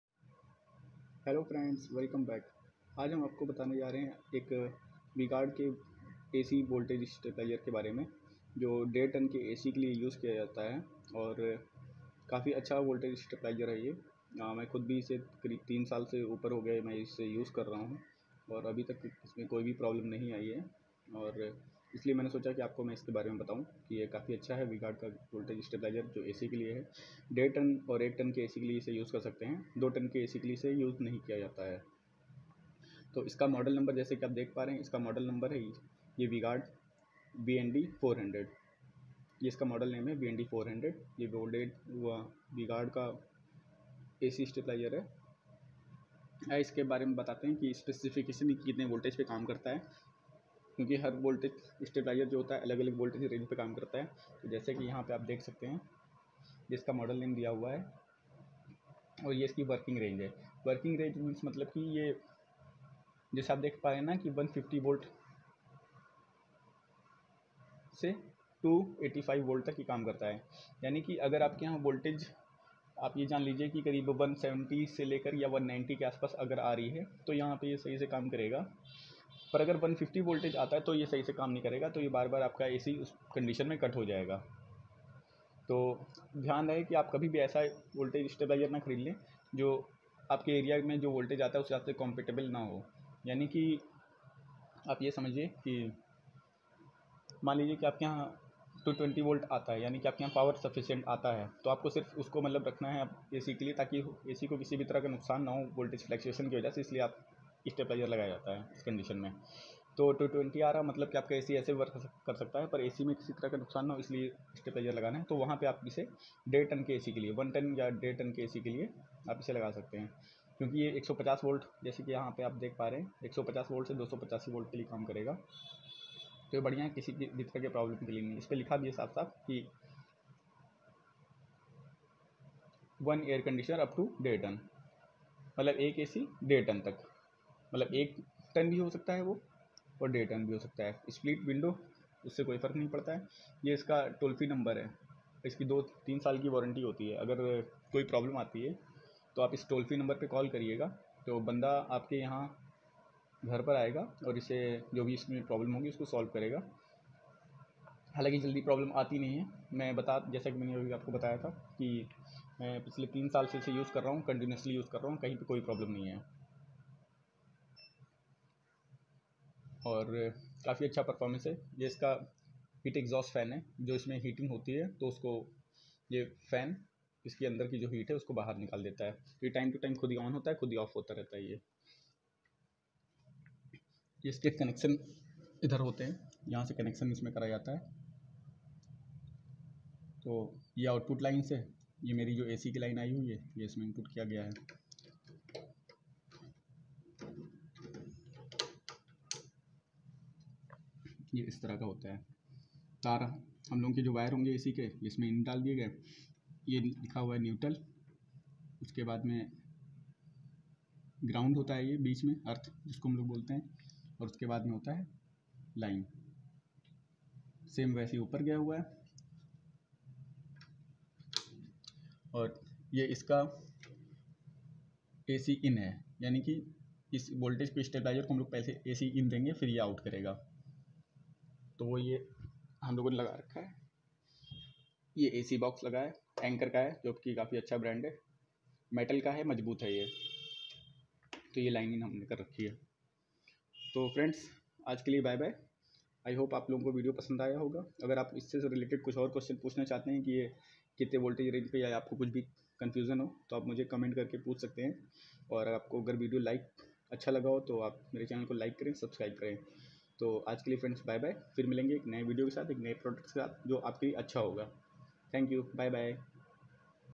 VGuard AC Stablizer for upto 1.5 ton Air Conditioner
V guard ac stabilizer
VND400

Warranty type - 3 yrs (home service)
Voltage range -150 to 285 volt AC. 50Hz
Upto 1.5 ton air conditioner split or window .

Hii friends thanks to all for support me.